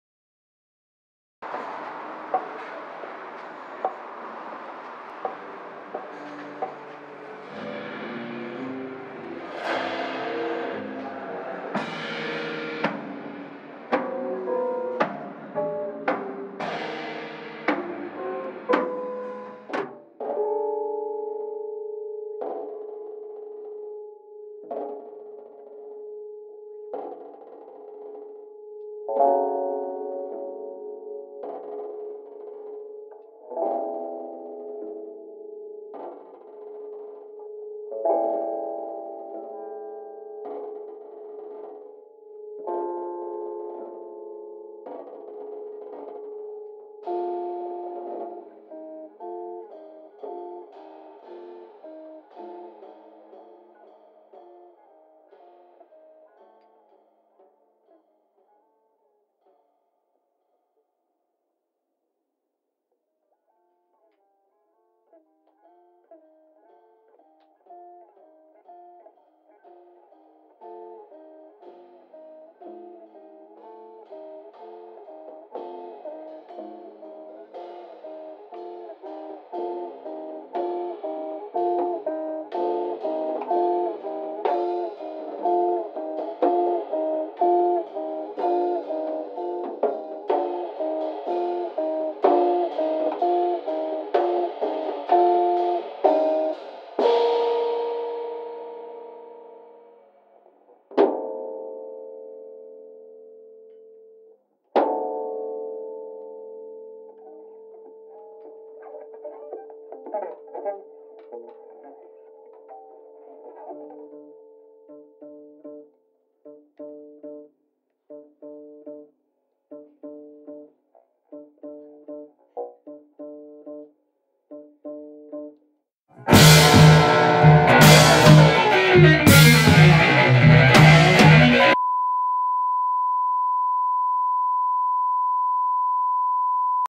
The Rite of Trio - How would you like a cup of TROT?
First of a series of 2 episodes advertising the upcoming show in Porta-Jazz

Special thanks to Alexandra Ribeiro, Henrique Lopes and Diana Martinez!

The Rite of Trio will be live this Saturday - 01/02/2014, 19h
Porta-Jazz - Edifício AXA, 4º Piso